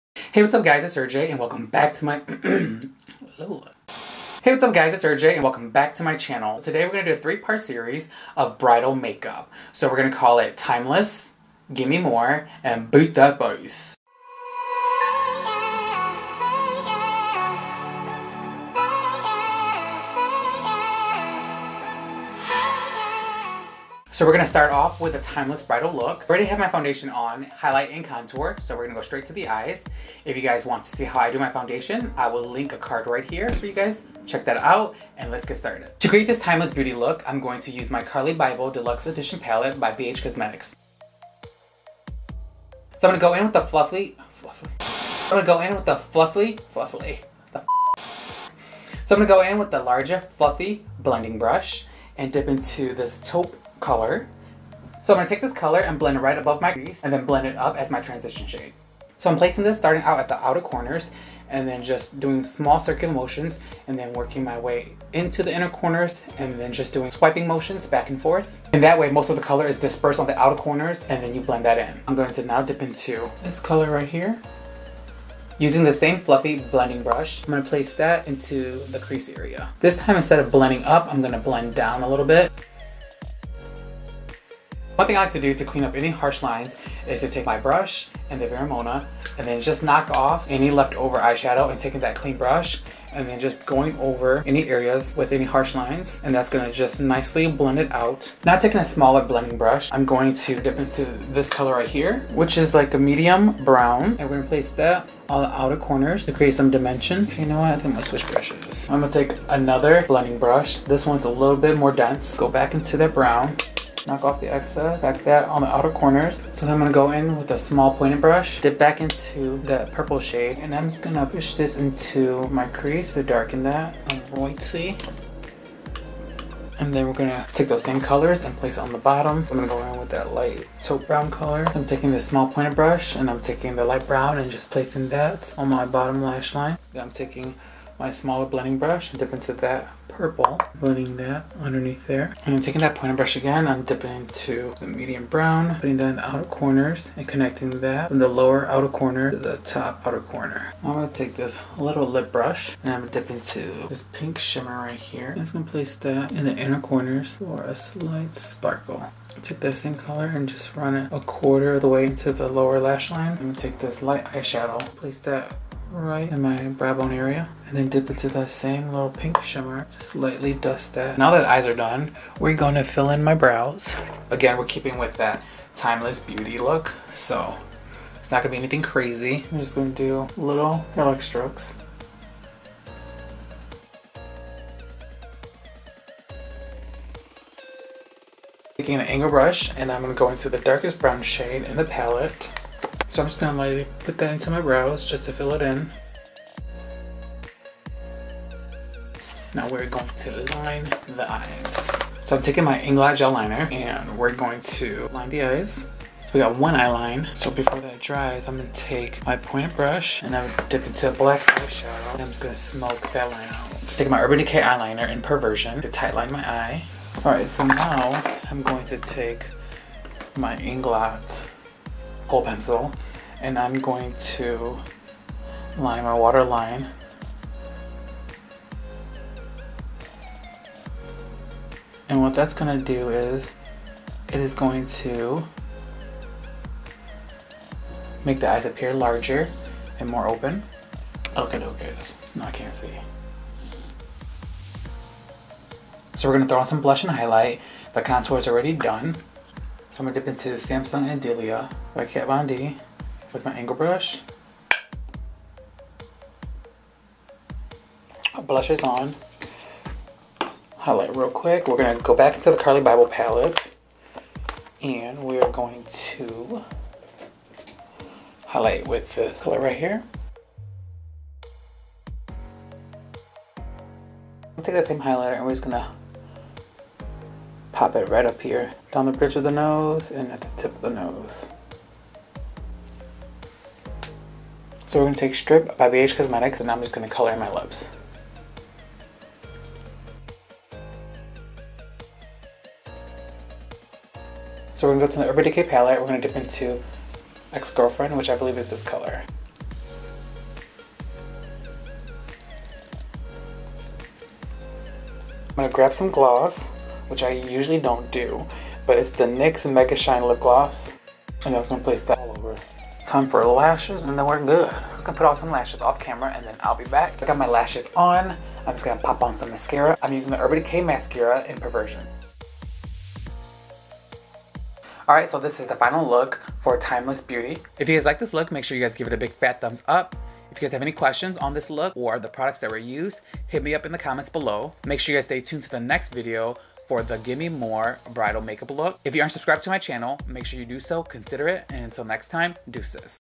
TIMELESS BRIDAL MAKEUP - PART 1 OF 3 LOOK SERIES | CHER JAY LEE
Welcome to part 1 of my 3 part Bridal Makeup Series! I wanted to provide my viewers, clients and potential clients a look of 3 possible bridal makeup options for the Big Day. In part 1, we cover the popular and most requested bridal makeup look, “Timeless Beauty.” A soft glam makeup look that enhances your natural beauty – leaving no regrets today, tomorrow or 10 years from now.

Thumbs Up if you enjoyed this “Timeless Beauty” makeup look!

*More Looks to Come*
Part 2 – “Gimmie More”
Part 3 – “Beat That Face”